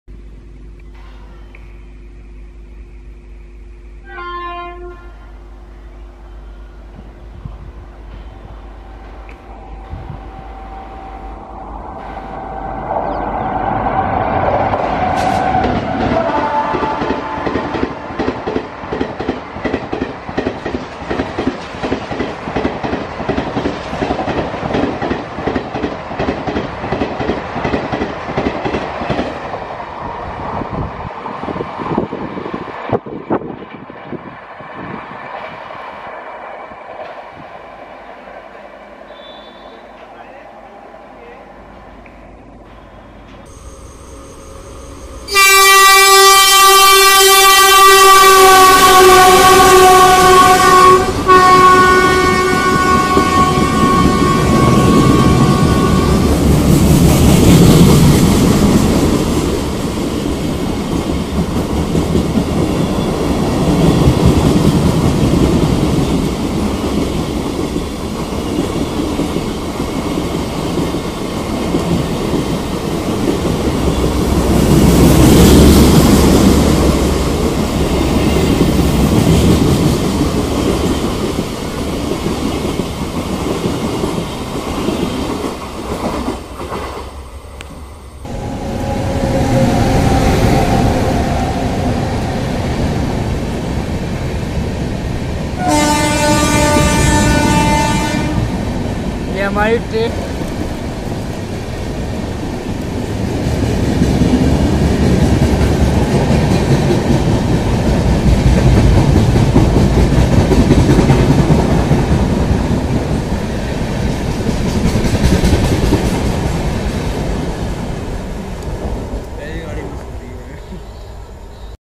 दूरंतो के अवतार मे हाइब्रिड डिब्बों के साथ हाथरस से पटरी तोड़ती निकली भुवनेश्वर आनंद विहार एक्सप्रेस
GZB WAP 7 WITH DURONTO LIVERY 02813 Bhubaneshwar Anand Vihar Terminal Weekly Express Skip Hathras Junction Flat 120 Kmph